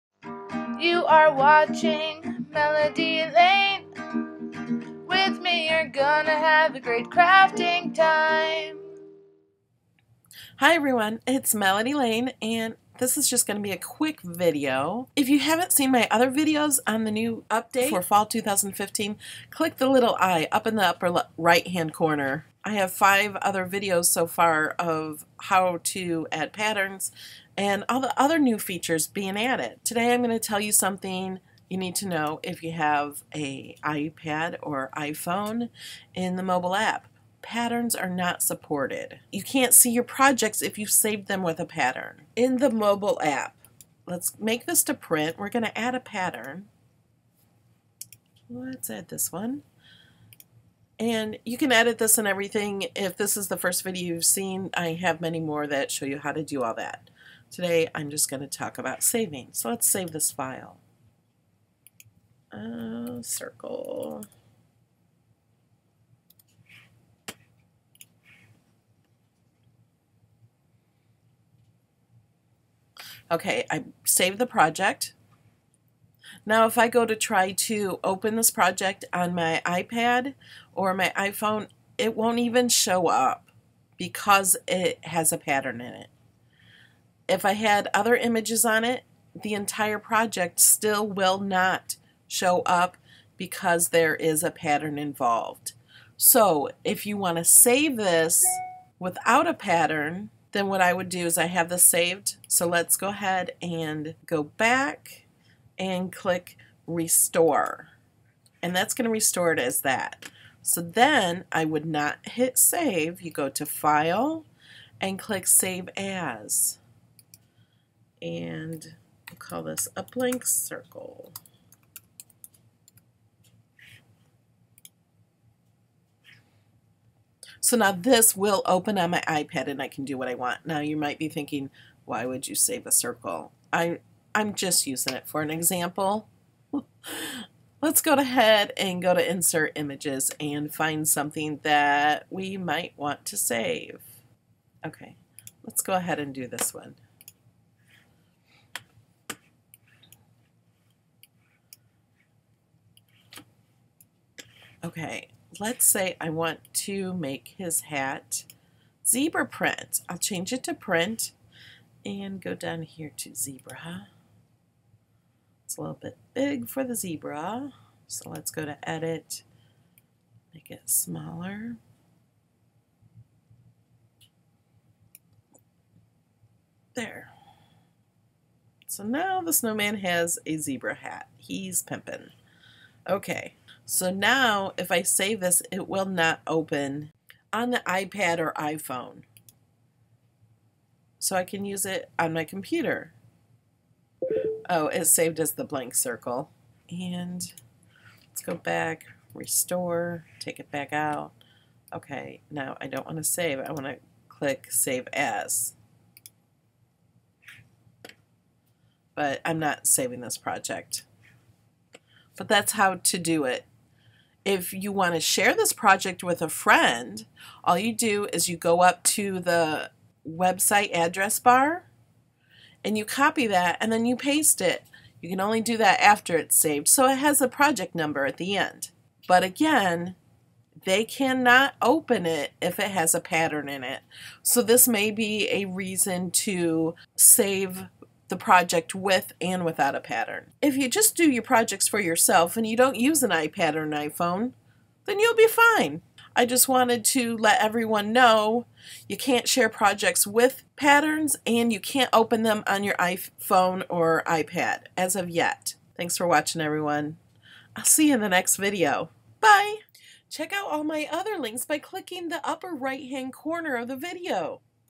Saving & Sharing Projects with Patterns in Cricut Design Space- Beta Preview
Learn how you can and can NOT share projects with patterns in Cricut Design Space.

Melody Lane
640 N Greece Rd